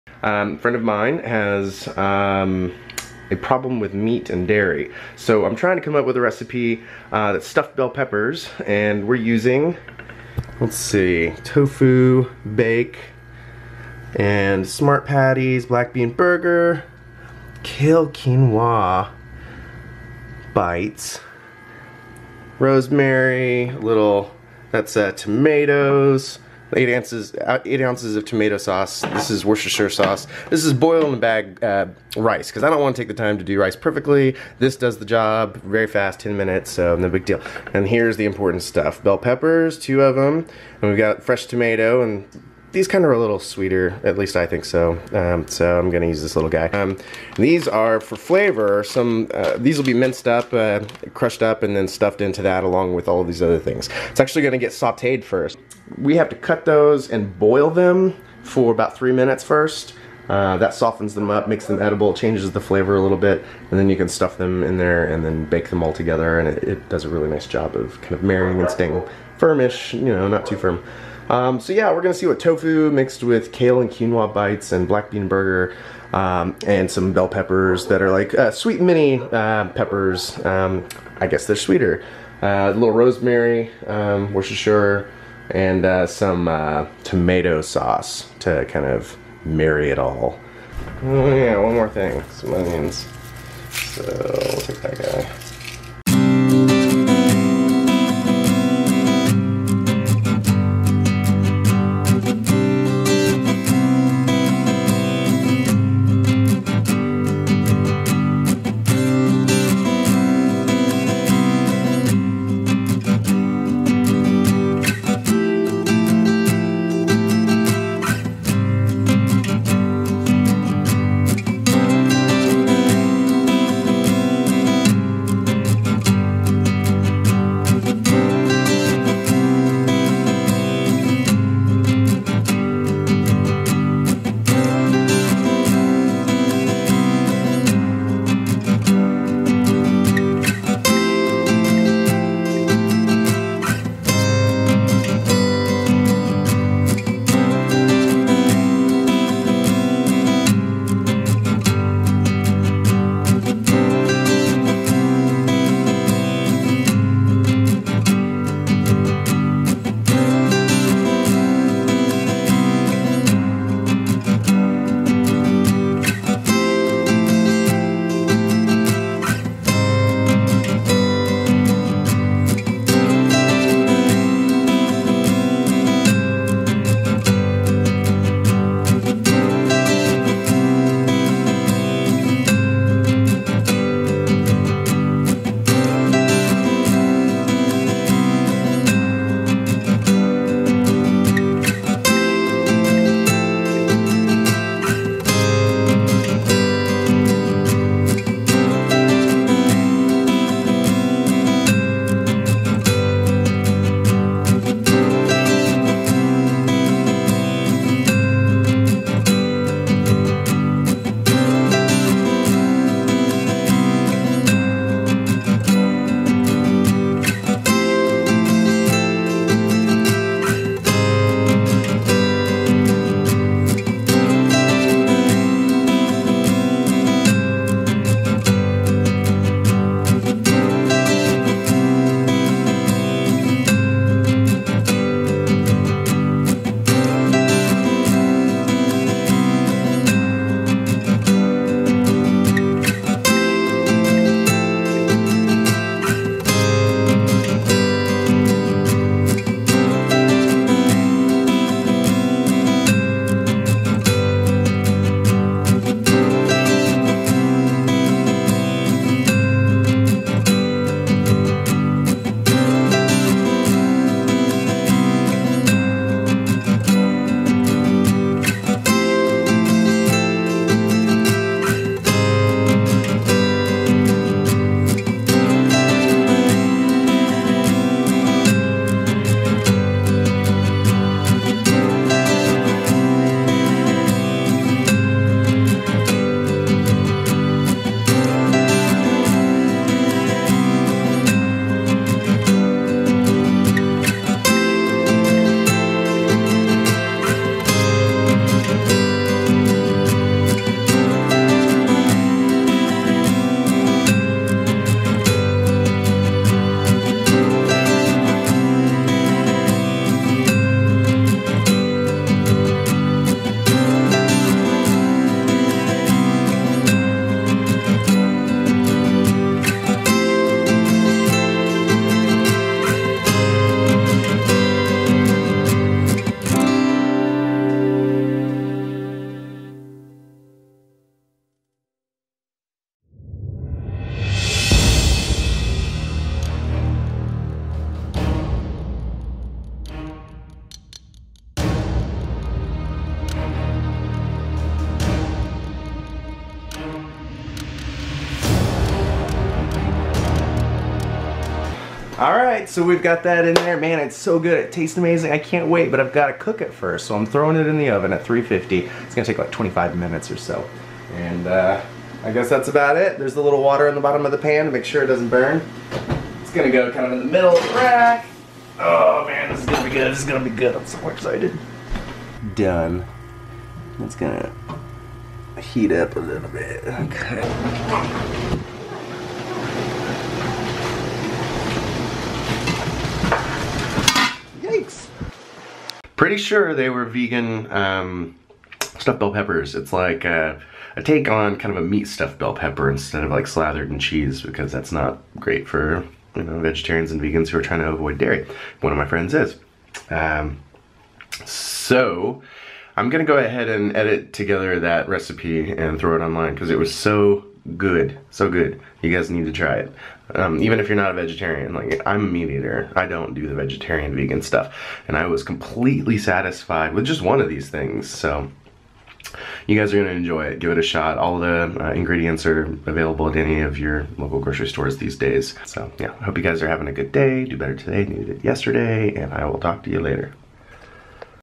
BS Vlog 026 Vegan Stuffed Bell Peppers
A break from interviews and life in the city to make vegan stuffed bell peppers for a friend